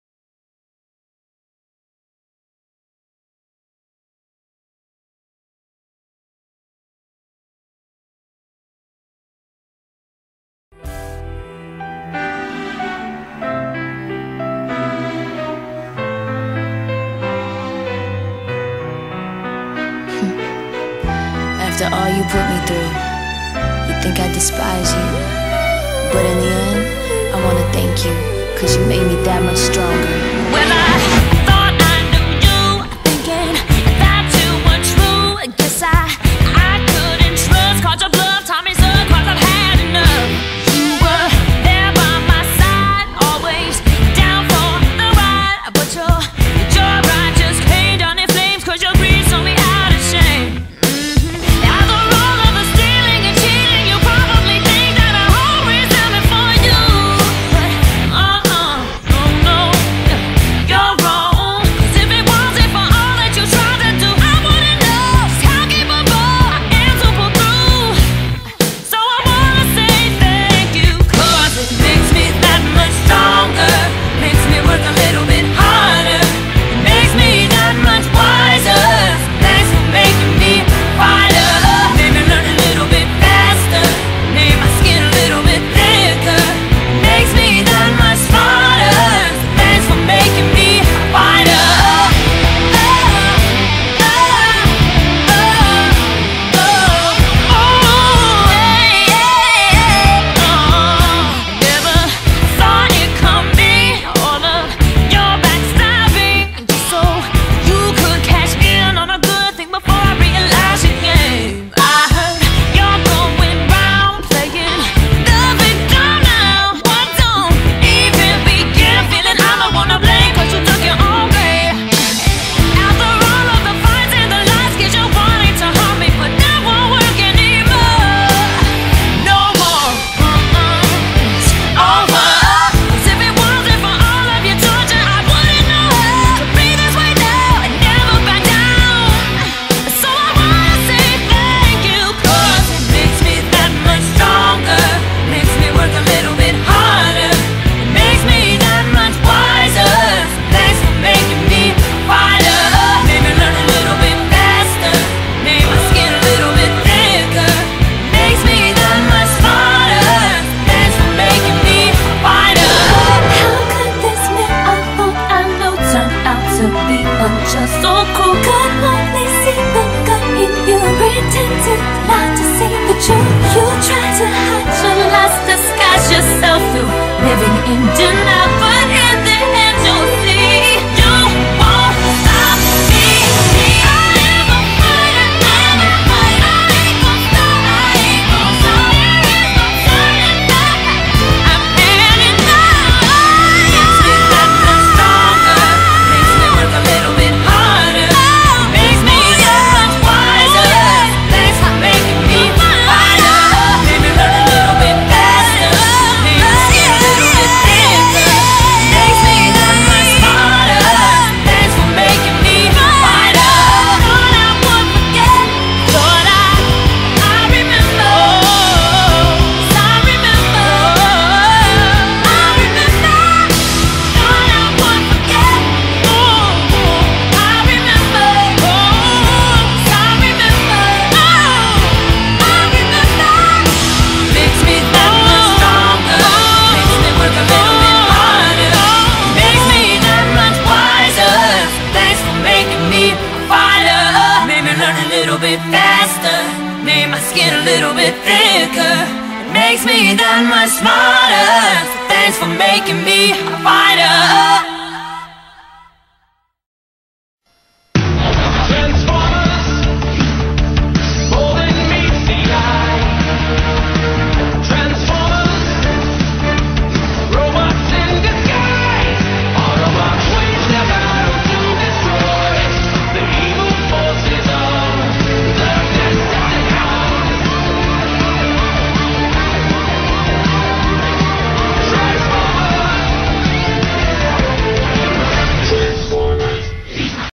Blackarachnia Fighter
This is a tribute to Animated Blackarachnia well the songs really fits with her